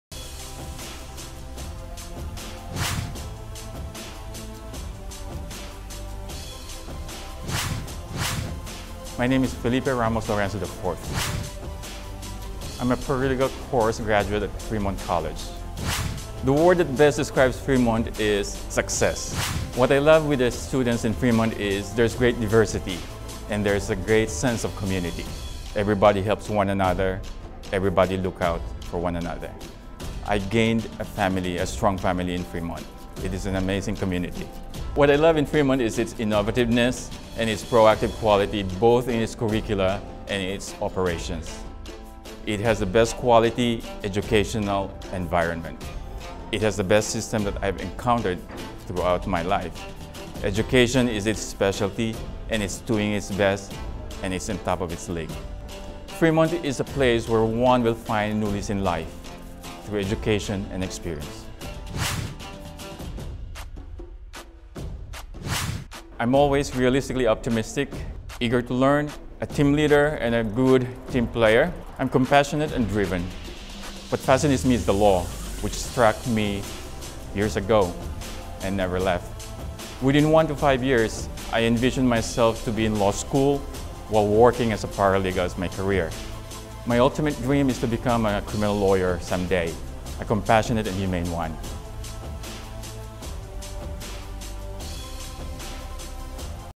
Fremont College Graduate: Felipe Lorenzo - Paralegal Studies
Fremont College proudly presents College of Legal Studies graduate Felipe Lorenzo. Felipe talks about the strong sense of community at Fremont College as well as his dreams of becoming a criminal lawyer .